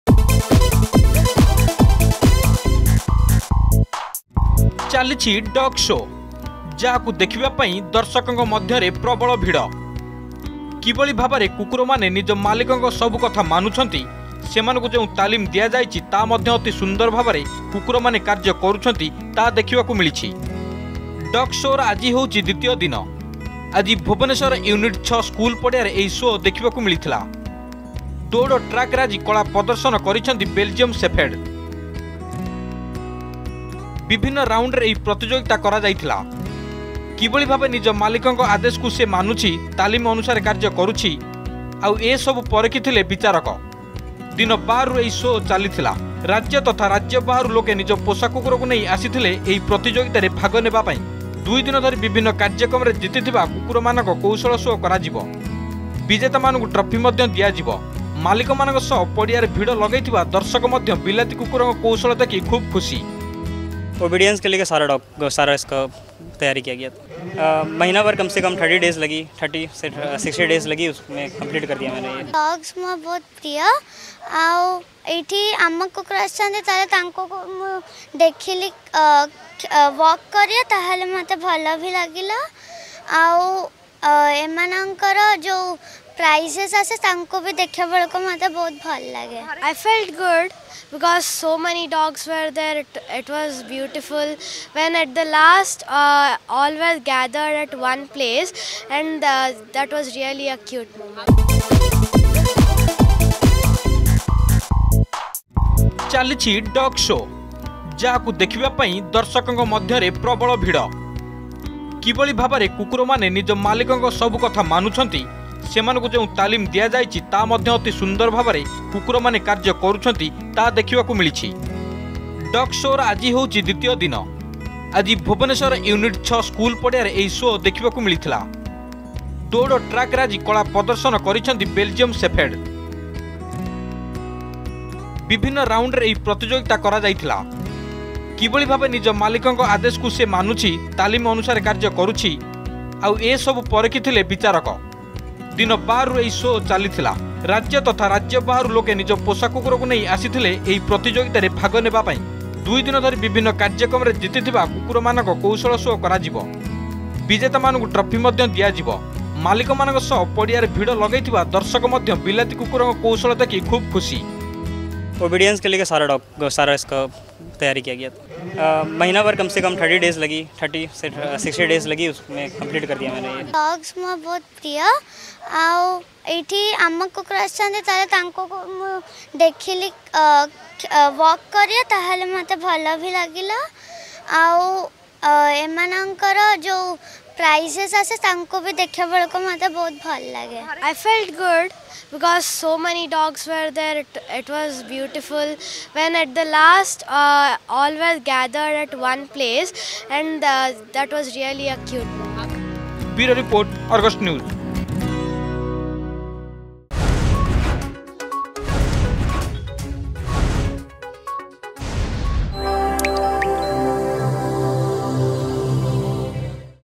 ଦୌଡ଼ ଟ୍ରାକରେ ଆଜି କଳା ପ୍ରଦର୍ଶନ କରିଛନ୍ତି ବେଲଜିୟମ ସେଫଡ
ଚାଲିଛି ଡଗ ସୋ। ଯାହାକୁ ଦେଖିବା ପାଇଁ ଦର୍ଶକଙ୍କ ମଧ୍ୟରେ ପ୍ରବଳ ଭିଡ। କିଭଳି ଭାବରେ କୁକୁରମାନେ ନିଜ ମାଲିକଙ୍କ ସବୁ କଥା ମାନୁଛନ୍ତି, ସେମାନଙ୍କୁ ଯେଉଁ ତାଲିମ ଦିଆ ଯାଇଛି ତାହା ମଧ୍ୟ ଅତି ସୁନ୍ଦର ଭାବରେ କୁକୁରମାନେ କାର୍ଯ୍ୟ କରୁଛନ୍ତି ତାହା ଦେଖିବାକୁ ମିଳିଛି ।